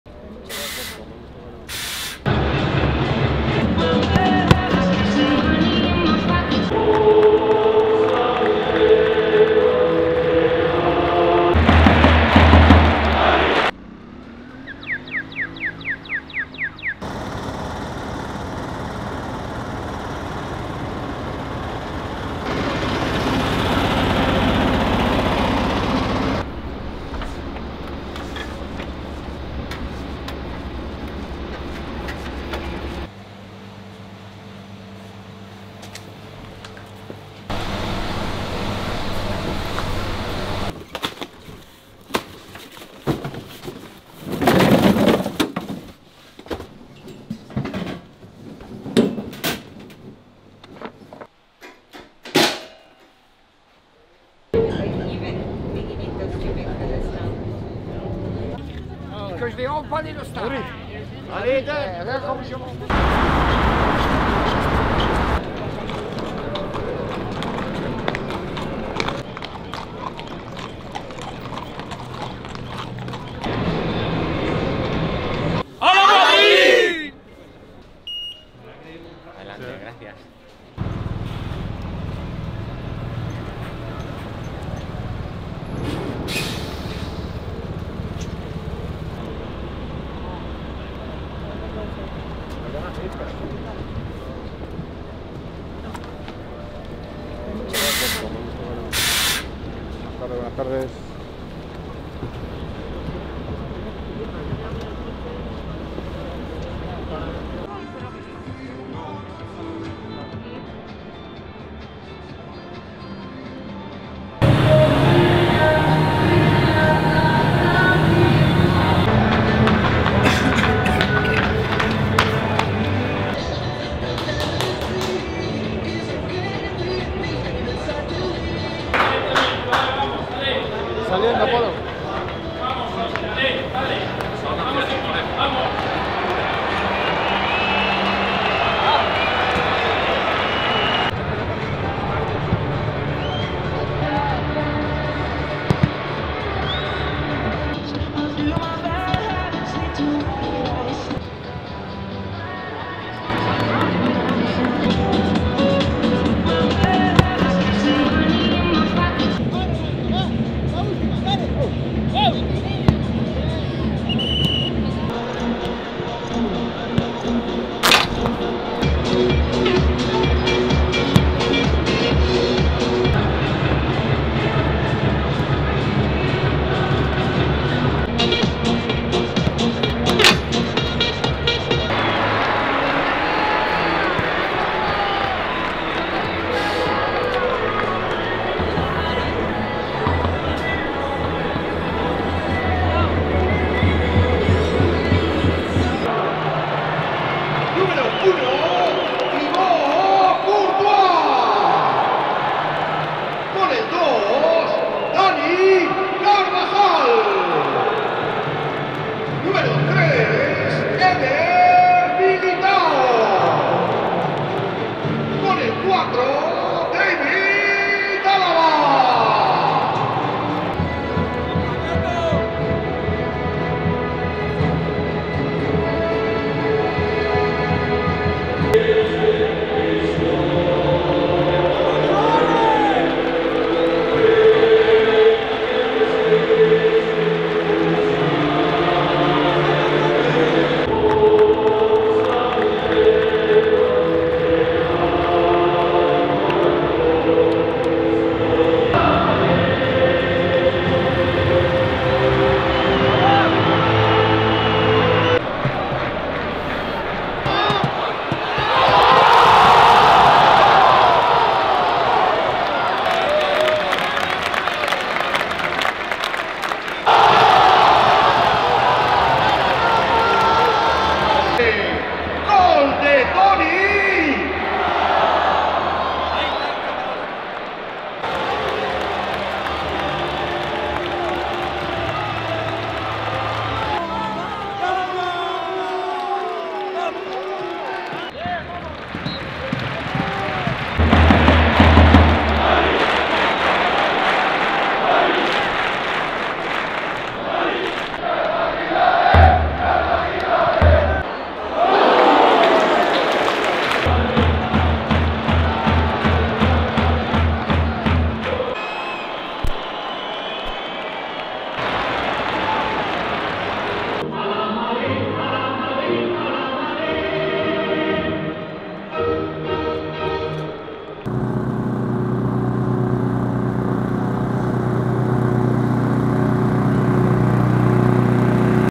The SOUND of FOOTBALL at the BERNABÉU! | Real Madrid
There's nothing like the sound of football at the Bernabéu when Real Madrid are playing at home! Experience all the sounds of a typical matchday at home at the Santiago Bernabéu in our latest video. See how the Santiago Bernabéu stadium pitch was prepared, the arrival of the players and much more before and during Real Madrid's recent LaLiga match against Rayo Vallecano.